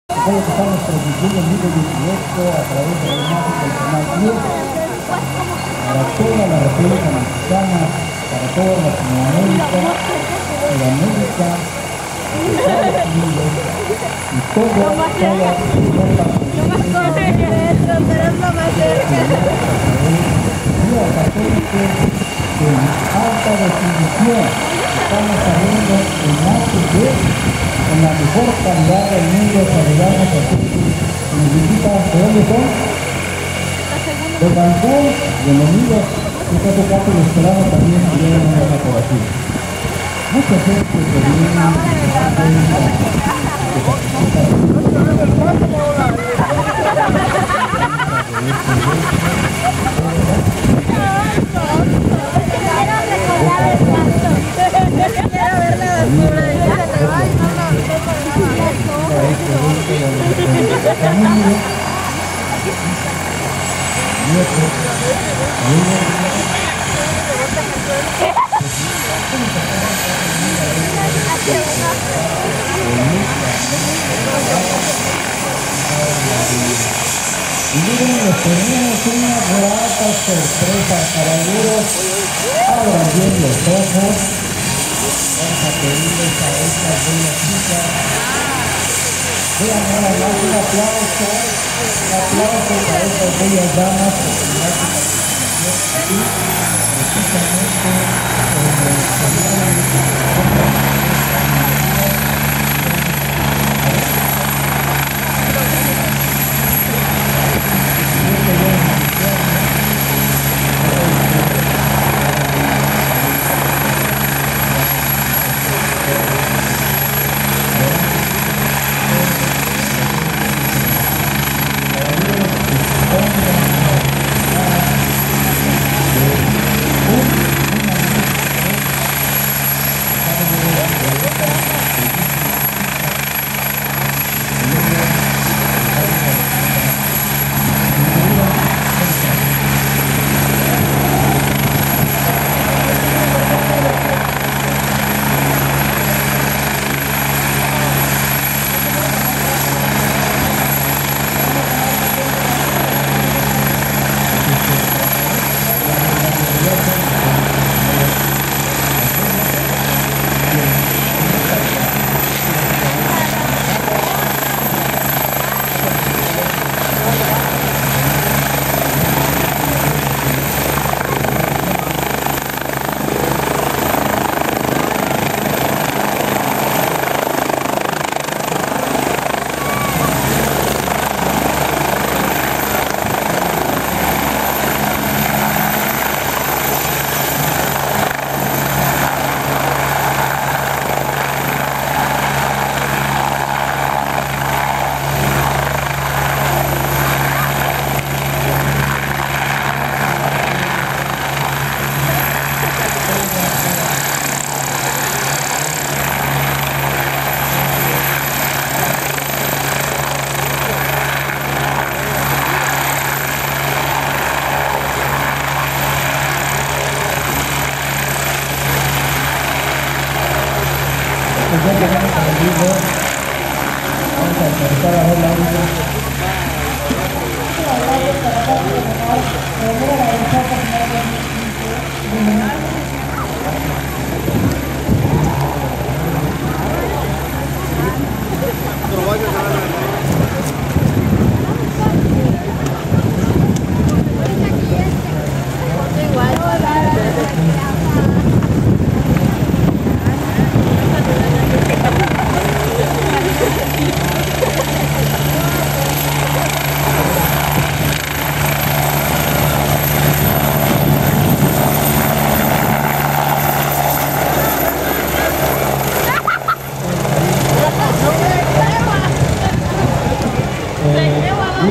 AgustaWestland AW109 Power at Aeroshow Cozumel - Mexico
El Gobernador de Quintana Roo en Aeroshow Cozumel - México